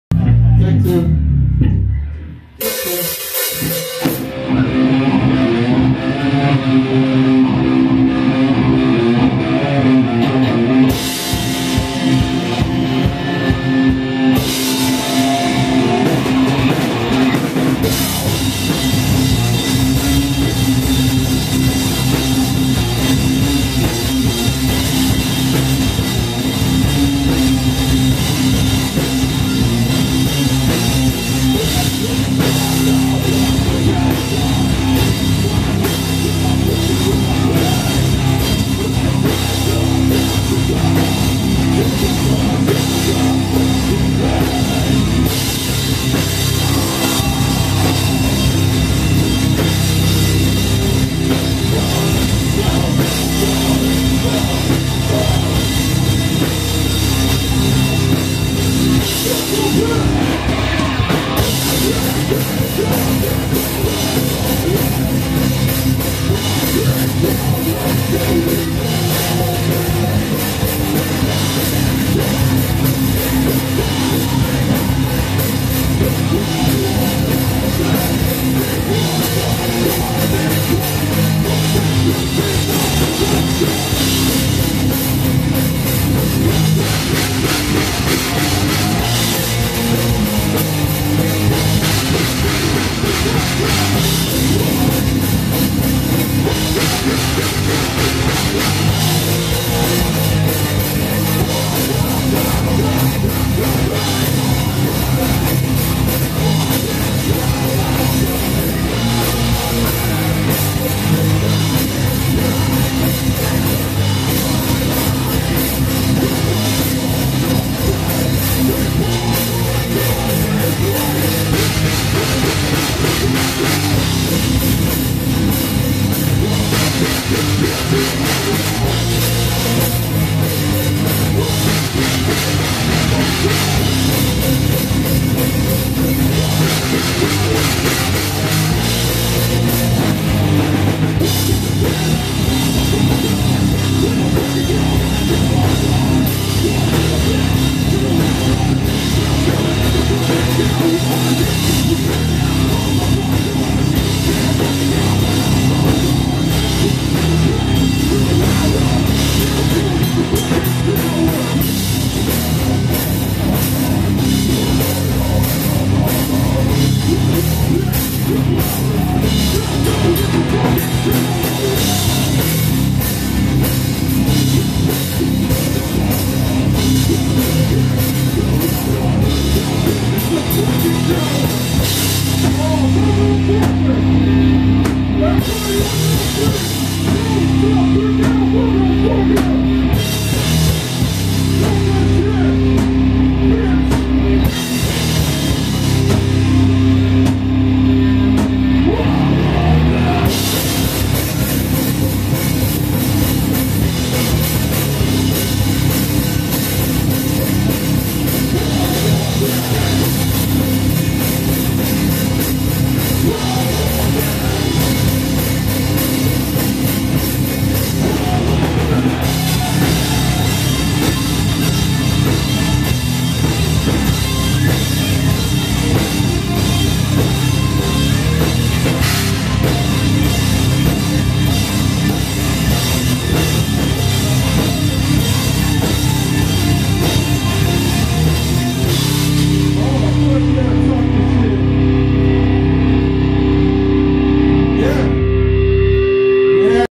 Band Practice - Judgement Day (9-23-2019)
Here's some footage from one of our first band practices with Johnny (our bass player) back when the band was first forming. 🤟